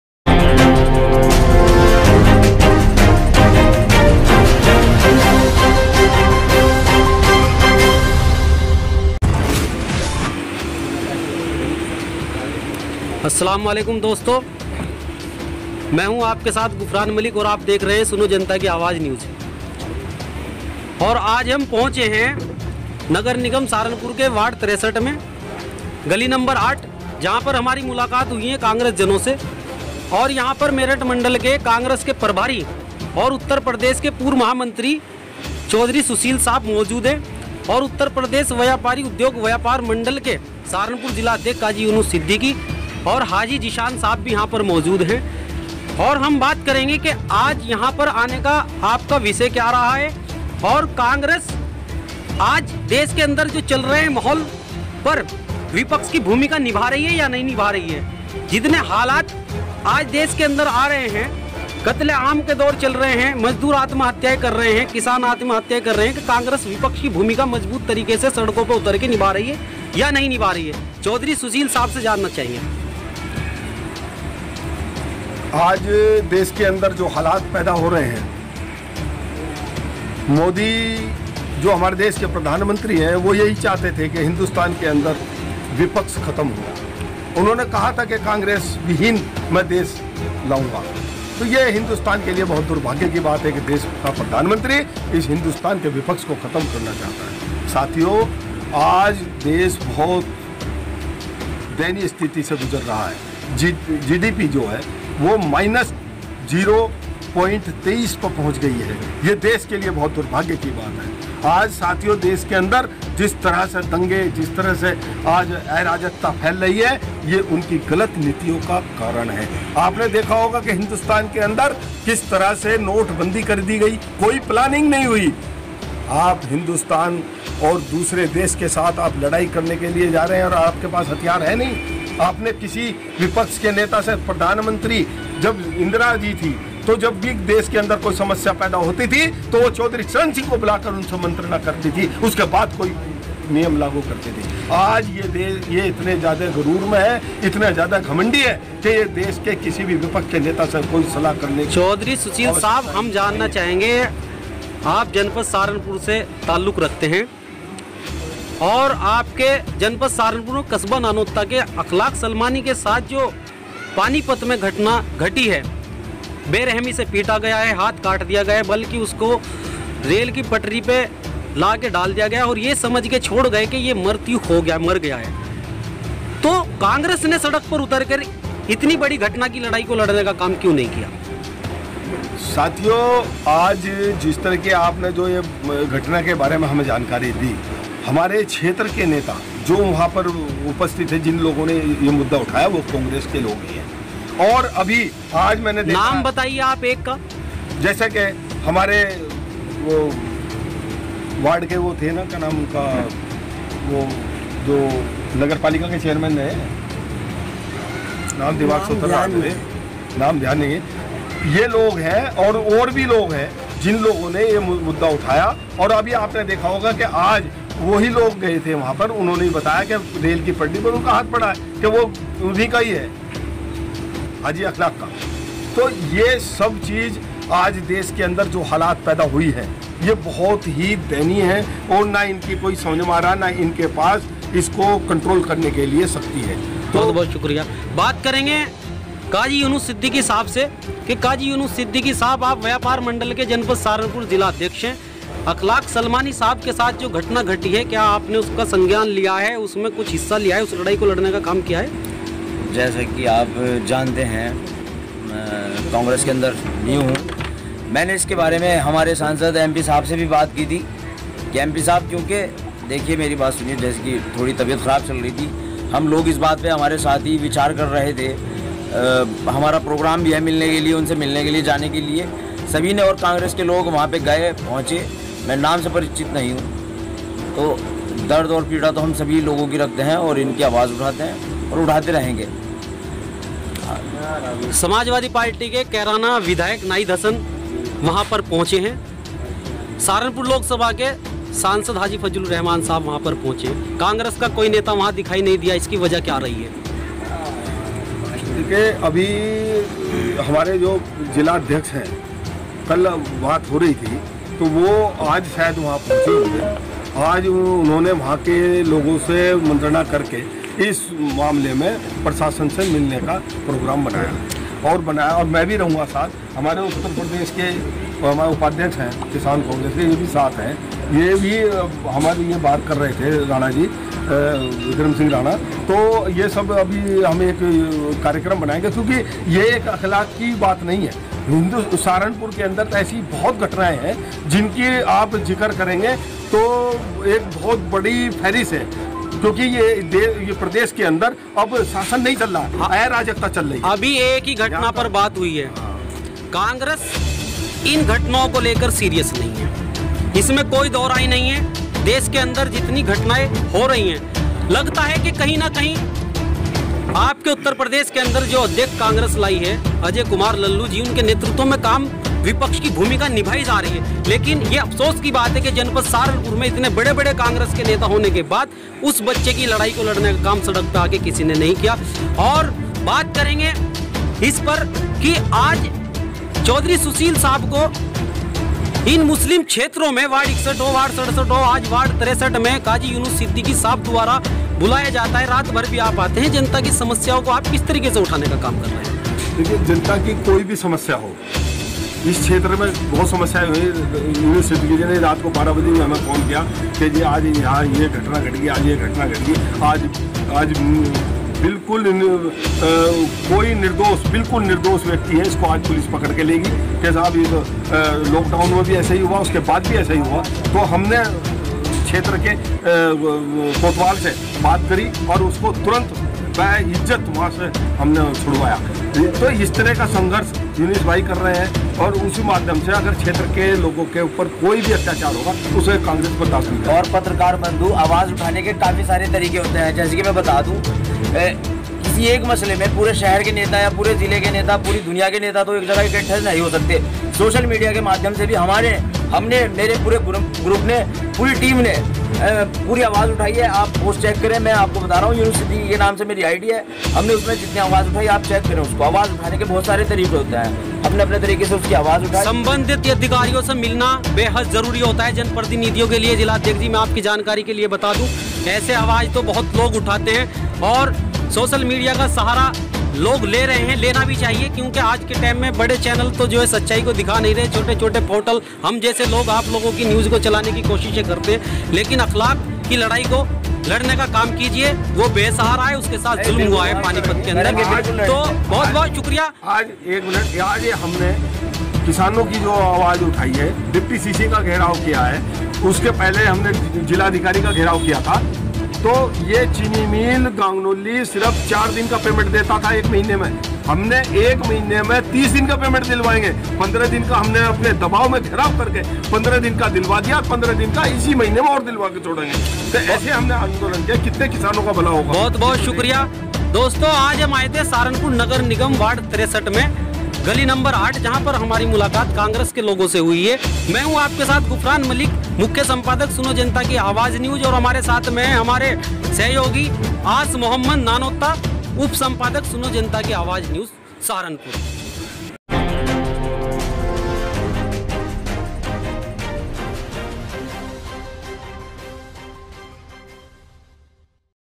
कांग्रेस नेता चौधरी सुशील जी एवं यूनुस सिद्दीकी ज़ी से नानौता अखलाक पीड़ित को लेकर किए तीखे सवाल
दिनांक/18/9/2020/को
सहारनपुर नगर निगम वार्ड 63 की गली नंबर 8 में नदीम सिद्दीकी के आवास पर कांग्रेस पार्टी की एक मीटिंग का आयोजन किया गया है मीटिंग को संबोधित करते हुए प्रदेश कांग्रेस कमेटी के पूर्व महामंत्री मेरठ मंडल के प्रभारी सुशील चौधरी कमबोली एवं युवा कांग्रेस नेता काजी यूनुस सिद्दीकी ने कहा है की देशभर में कांग्रेसी आम जनता की लड़ाई लड़ रही है भाजपा सरकार की गलत नीतियों का विरोध संपूर्ण भारत में मात्र एक कांग्रेस पार्टी है जो कर रही है सुशील चौधरी ने यह भी कहा है कि भाजपा की तानाशाही किसी भी सूरत में बर्दाश्त नहीं की जाएगी और ना ही सरकारी विभागों का निजीकरण बर्दाश्त किया जाएगा

विक्रम राणा
सैयद आसिफ
आमिर
सादिक अंसारी
राशिद खान
फैसल अमीर
आहद हसन
सलीम मलिक
फरमान मलिक
राशिद खान
शहजाद मलिक
दानिश मिर्जा
आदि मौजूद रहे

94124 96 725